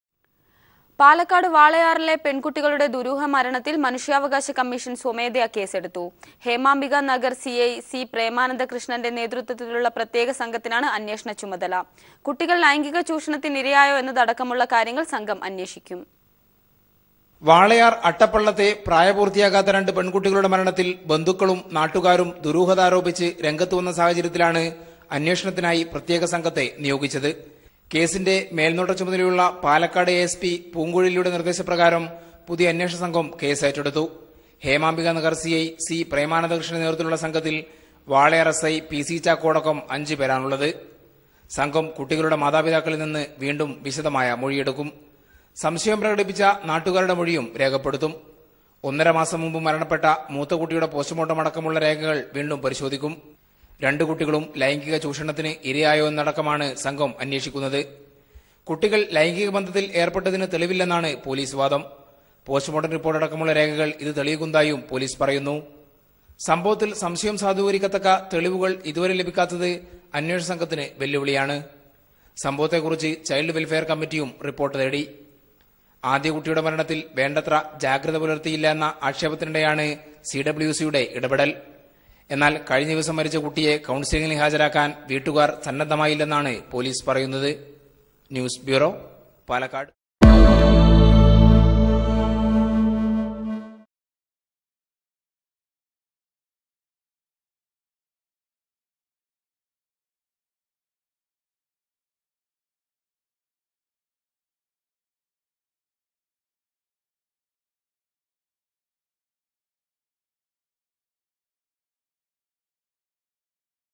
Human Rights Commission Voluntarily Takes Case In Girls' Mysterious Death In Walayar
In the mysterious death of grils in Walayar, Human Rights commission has filed  case voluntarily.